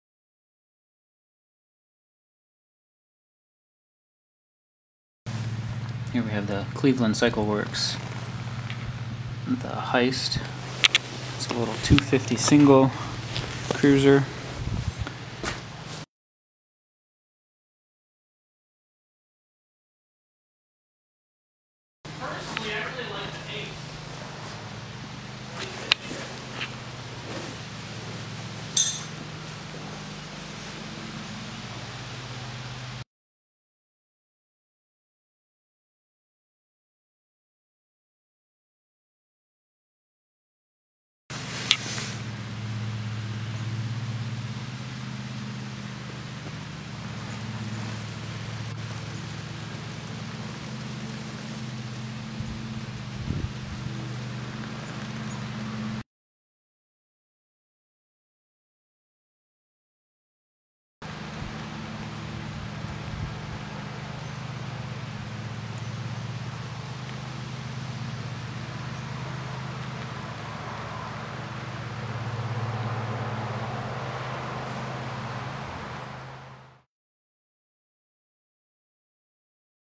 Cleveland CycleWerks - Tha Heist
I saw this bike the last time I was getting my Ural serviced at Scrambler Cycle. It is nice to see a low-cost ($3195 MSRP) American motorcycle.

PS: I didn't buy it, and unfortunately, I didn't get to ride it either